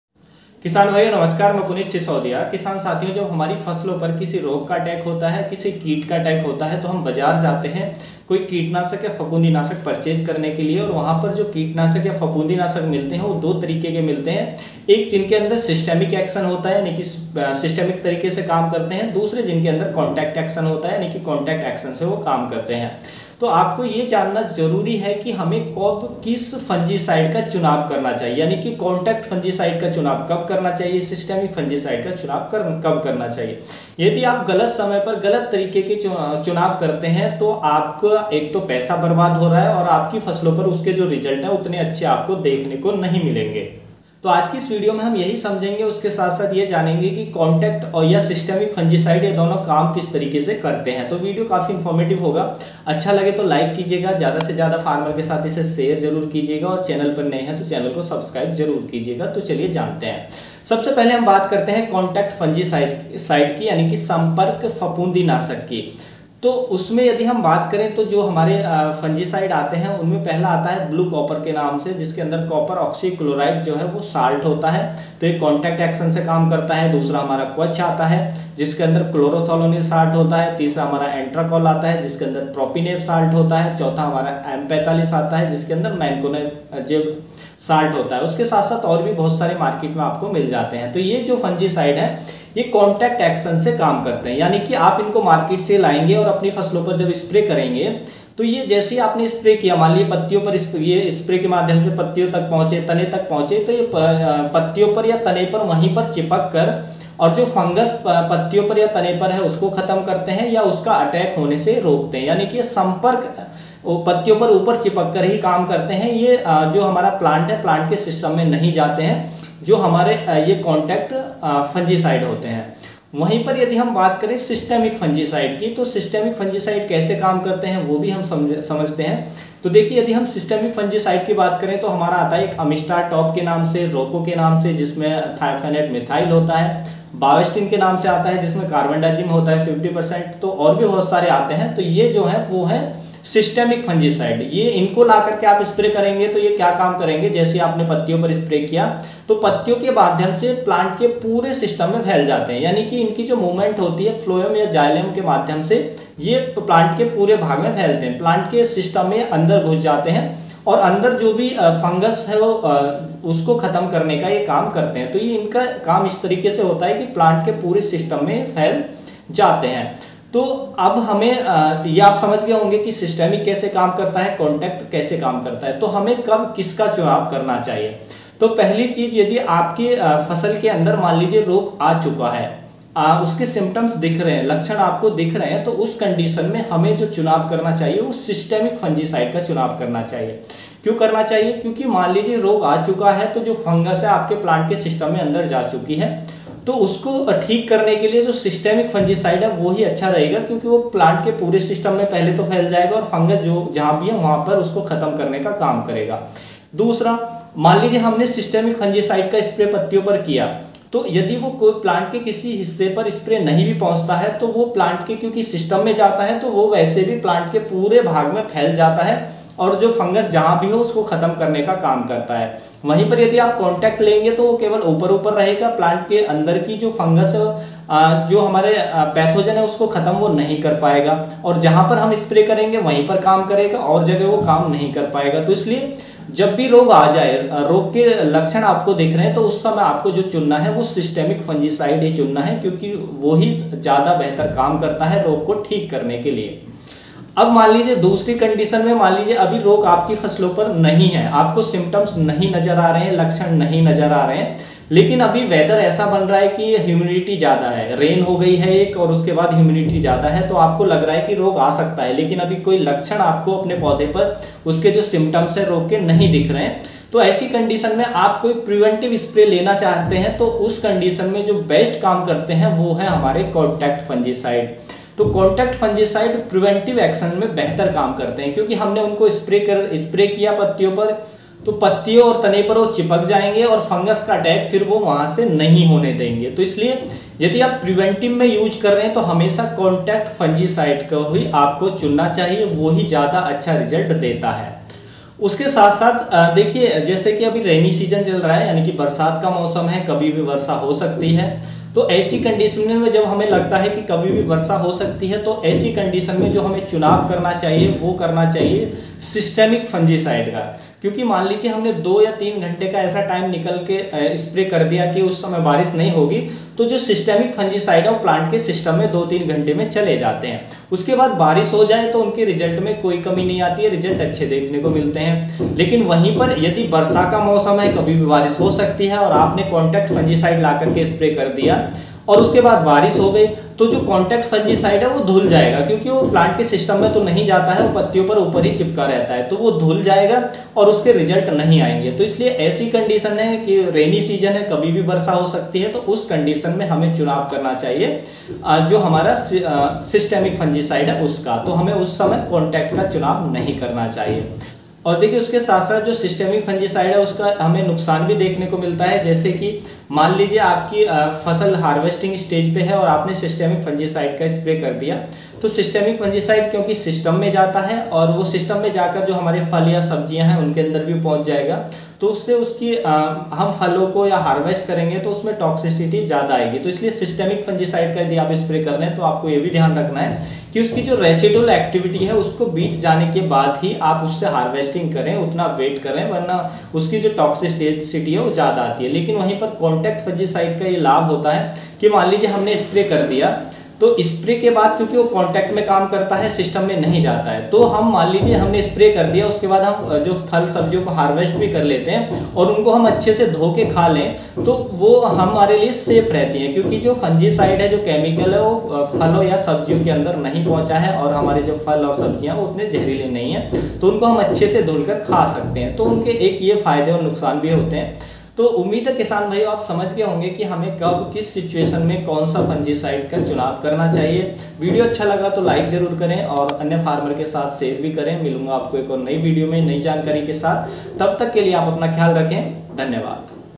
systemic या contact कब कौन सा | जान ले नहीं तो होते रहेंगे पैसे बर्बाद नहीं आएगा result
किसान साथियों फफूंदी नाशक दो तरीके के होते हैं | एक सिस्टमिक और दूसरा कांटेक्ट । लेकिन हमें कब कौन से फफूंदी नाशक का उपयोग करना चाहिए । यह जानना हमारे लिए बेहद जरूरी है। और systemic fungicide और contact fungicide कैसे काम करते हैं आज हम ये भी समझेंगे |

✅ Queries related -
(1) systemic fungicide
(2) contact fungicide
(3) how to choose best fungicide
(4) spray

Created by - punit shishodia

This video is education purpose only, not any promotion.